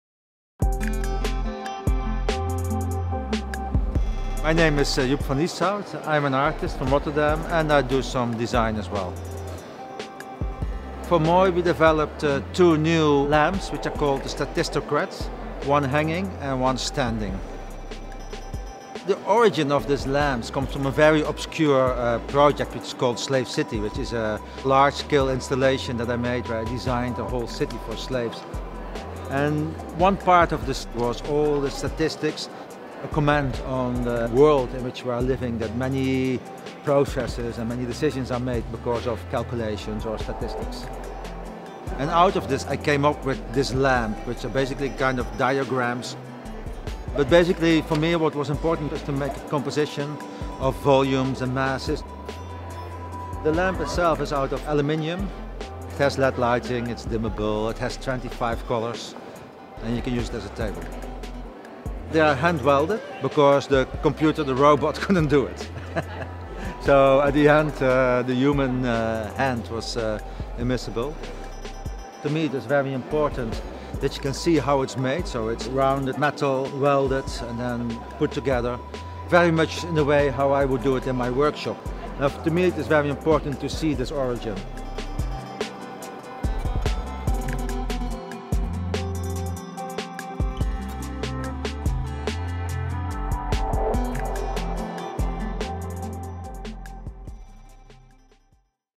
Statistocrat lamp by Atelier Van Lieshout for Moooi
In our next exclusive movie for Moooi, designer and artist Joep Van Lieshout explains how his Rotterdam-based studio came up with the branching form of its new Statistocrat lamp for the Dutch brand.

Launched at the Salone del Mobile furniture fair in Milan last year, Atelier Van Lieshout's Statistocrat lamp features a series of cylindrical shades, which branch off from a central metal pole.

Van Lieshout reveals in the movie that the lamp's distinctive form is derived from his studio's ongoing Slave City project, which consists of a series of artworks and installations based around an imaginary self-sustaining, zero-carbon prison city.

"The origin of this lamp comes from a very obscure project called Slave City," he explains. "It is is a large-scale installation where I designed a whole city for slaves."

As part of the project, Atelier Van Lieshout designed a number of diagrammatic structures featuring different volumes that represent different statistics.

"It's a comment on the world that we're living in, that many process and many decisions are made because of calculations and statistics," Van Lieshout says.

"Out of this, I came up with the lamp, which is basically a kind of diagram. It is a composition of volumes and masses."